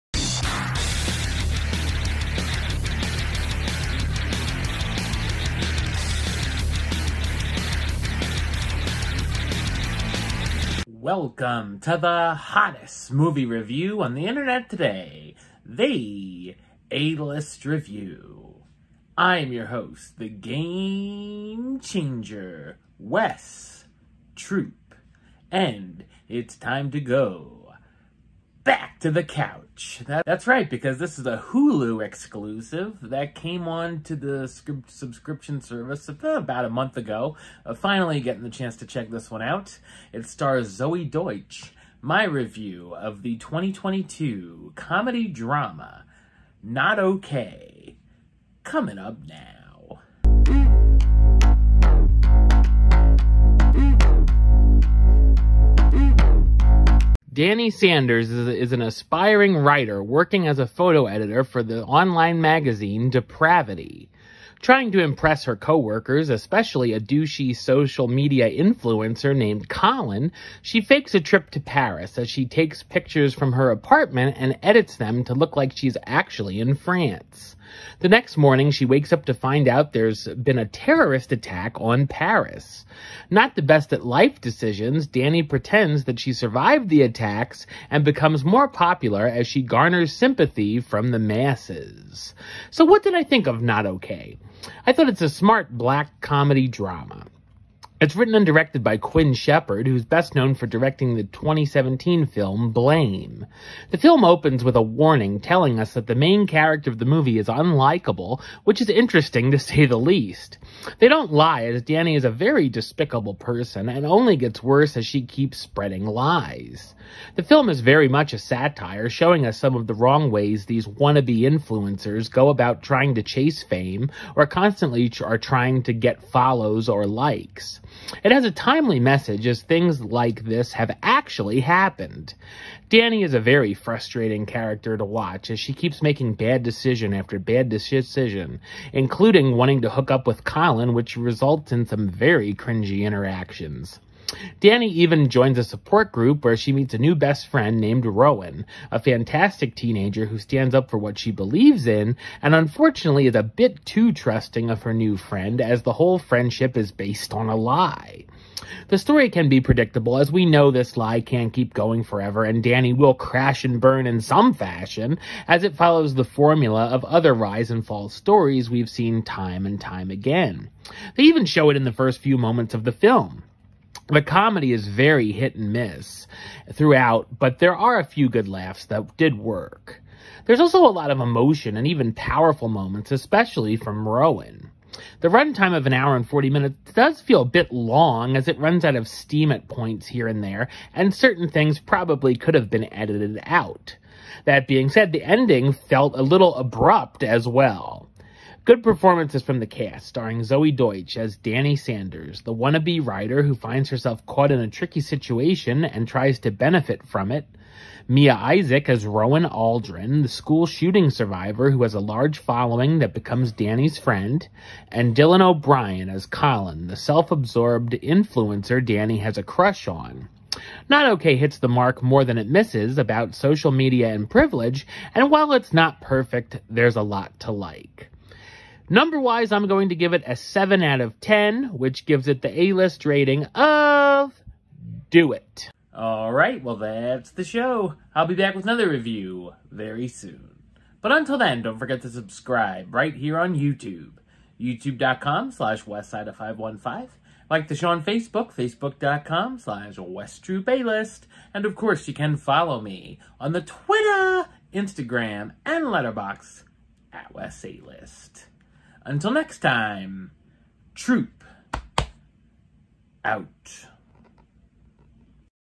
Not Okay - The A-List Review
In this episode, Wes reviews the 2022 comedy drama, Not Okay.

Follow Wes:
Instagram, Twitter, Letterboxd: @WesAlist